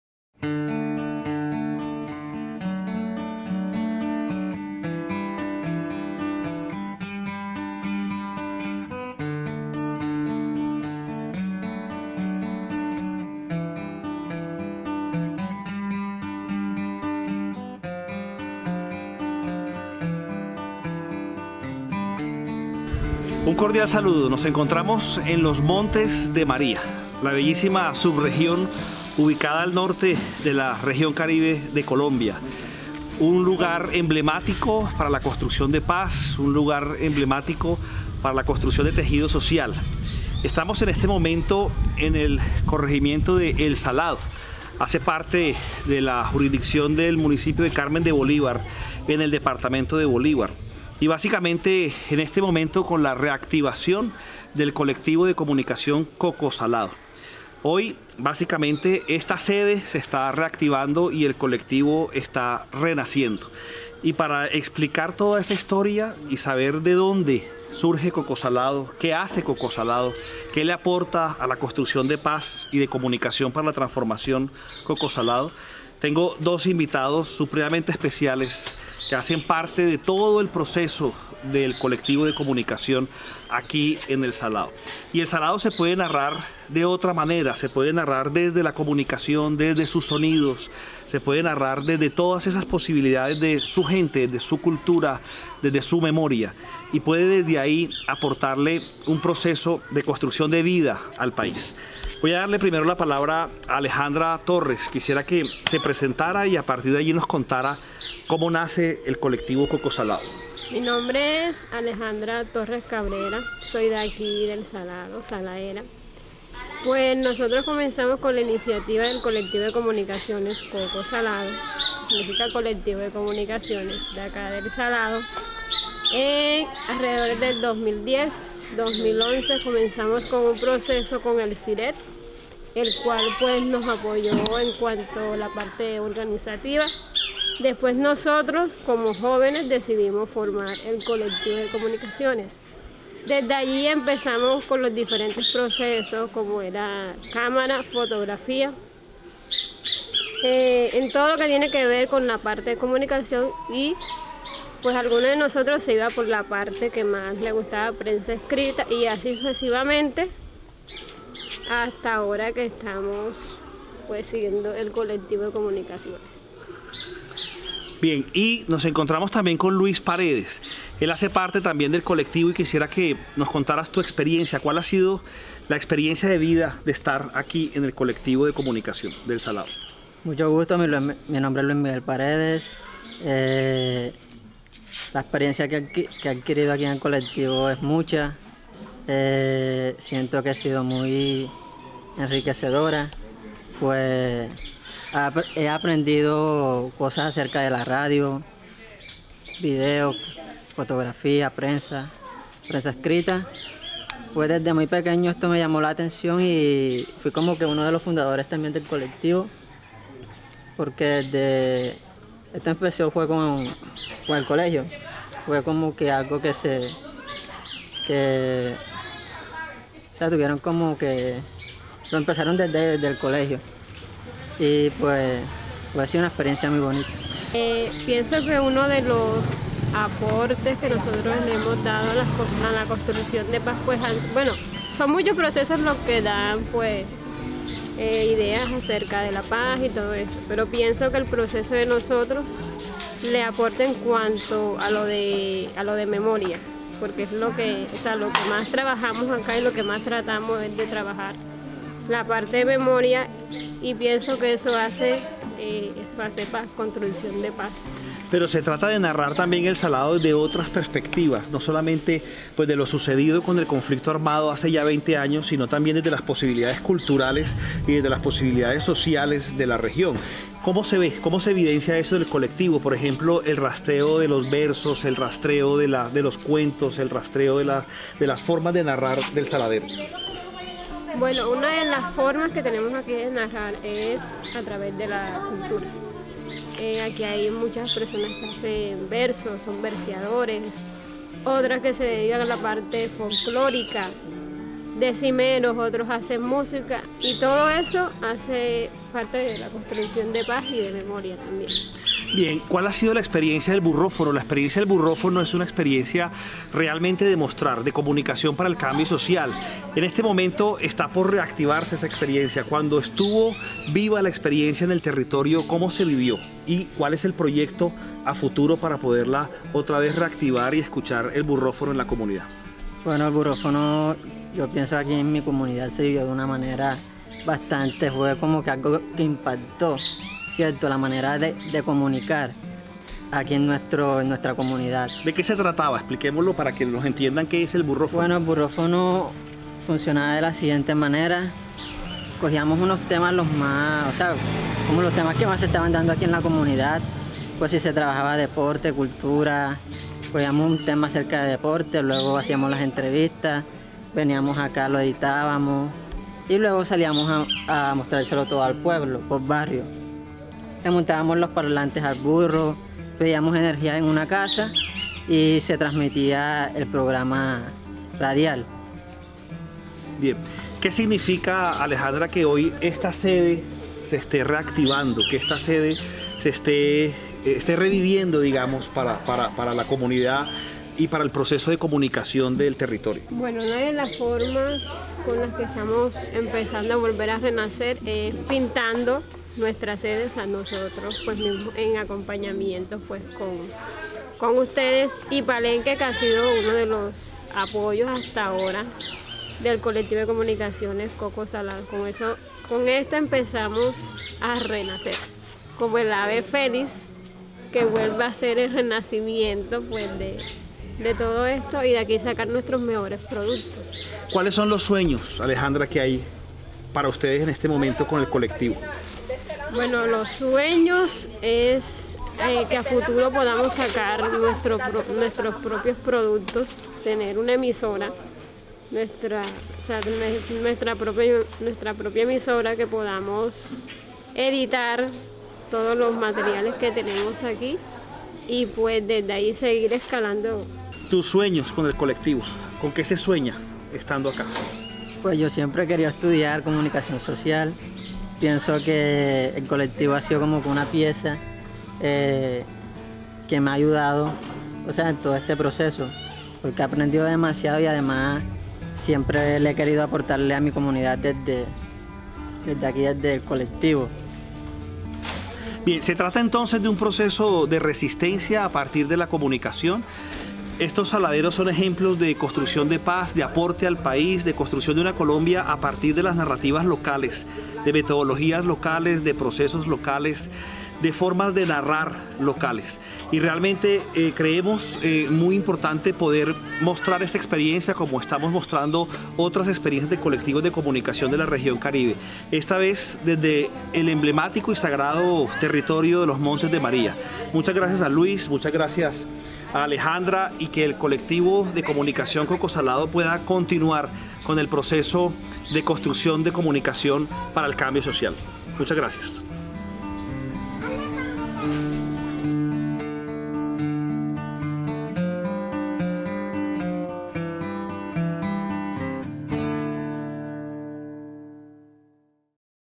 Experiencias del Colectivo de Comunicación Cocosalado: Volver al burrófono! (El Salado, Bolívar)
Procesos de reactivación del Colectivo de comunicación Cocosalado. Tod@s trabajando por la sede del colectivo, lugar de comunicación, lugar de sueños, lugar de proyectos. Queremos volver a escuchar los sonidos del burrófono recorriendo el pueblo con las historias de l@s salader@s. Rastreando experiencias de comunicación para el cambio social en el territorio!